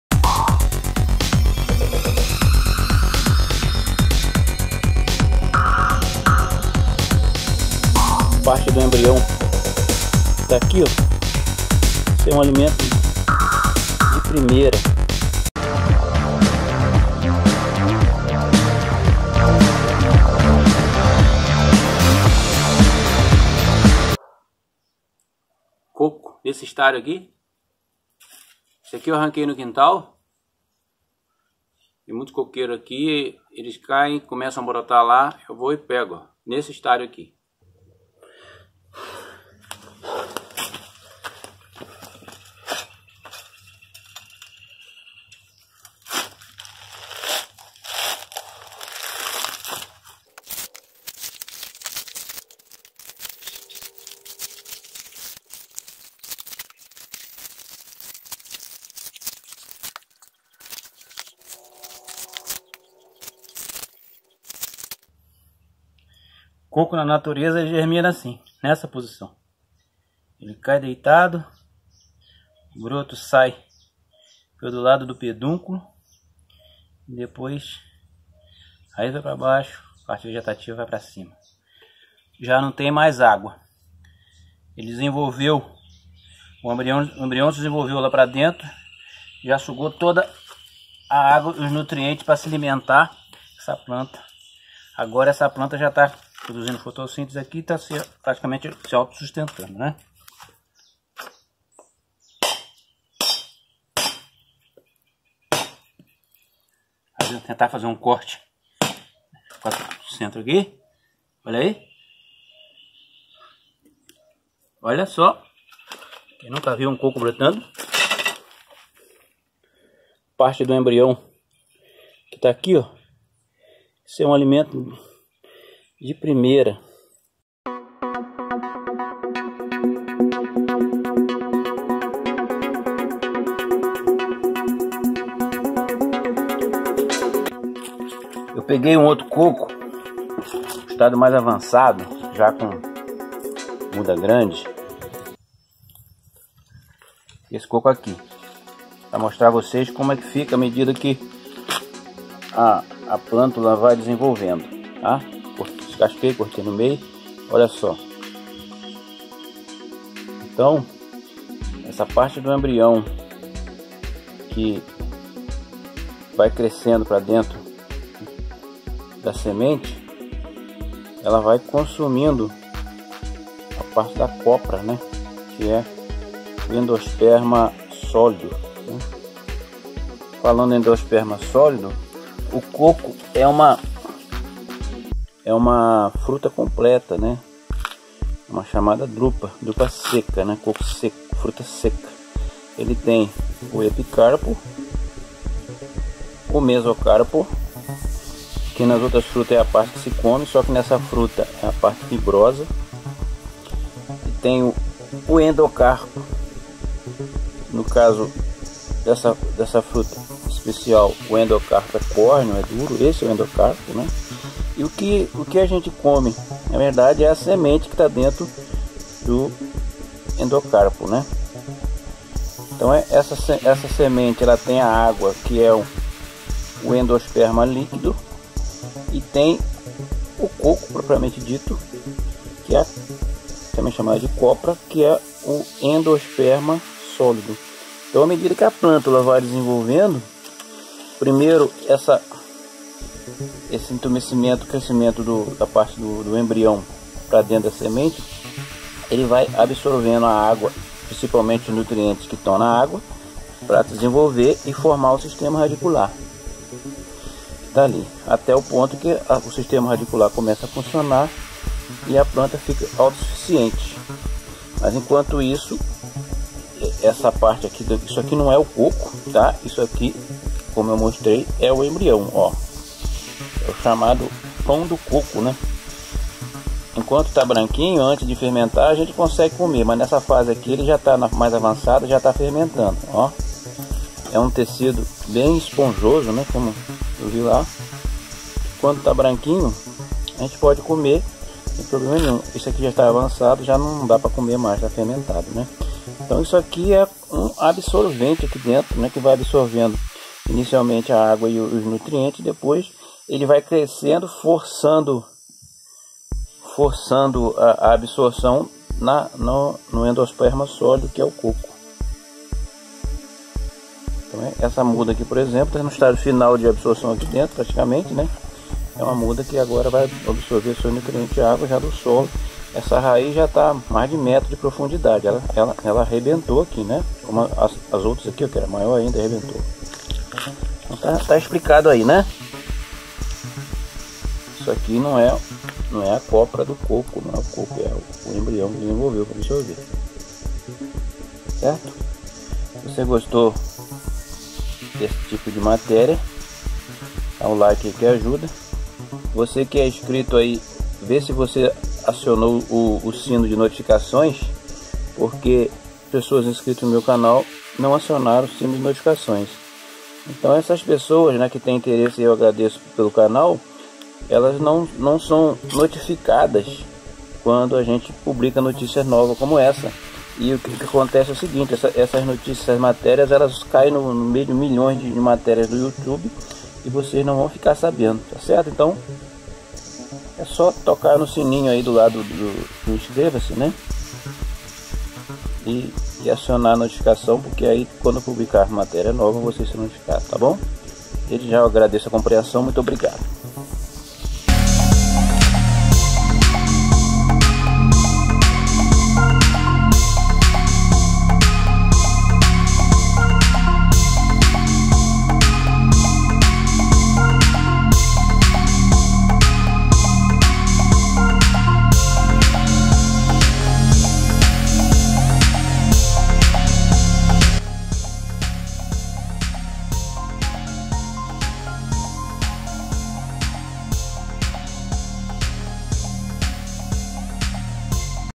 Entenda a morfologia e germinação do fruto do coqueiro Cocos nucifera Coconut fruit tree
Publicações em agronomia, fitotecnia, Fruticultura, horta, jardim, Fruteiras culinária caipira etc.